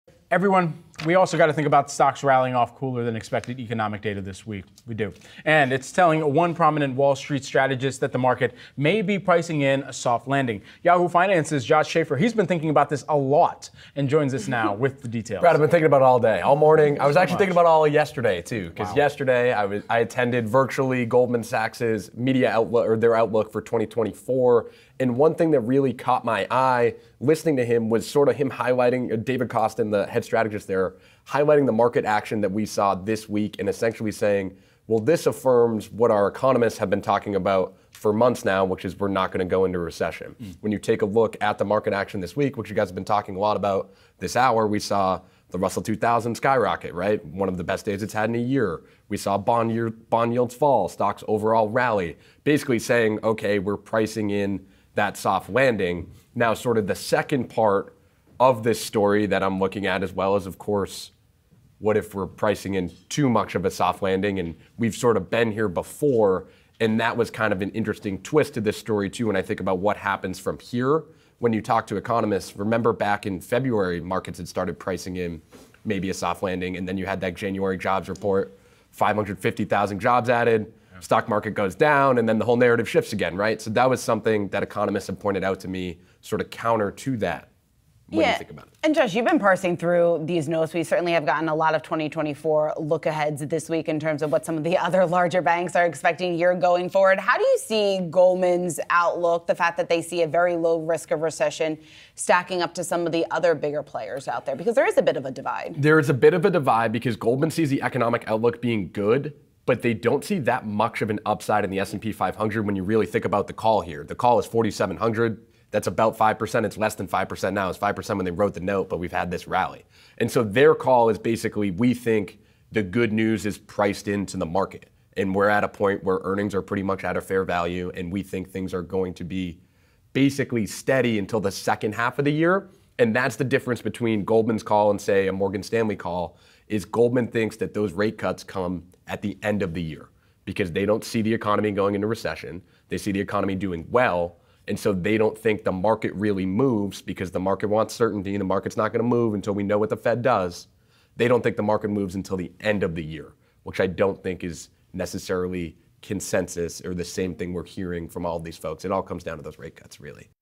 What the stock market rally says about the possibility of a soft landing
This week's stock market rally has Wall Street re-examine the potential for the US economy to pull off a soft landing scenario. Yahoo Finance Markets Reporter Josh Schafer breaks down Goldman Sachs' own outlook for 2024 and how the institution expects the economy to react to the Fed's monetary policy.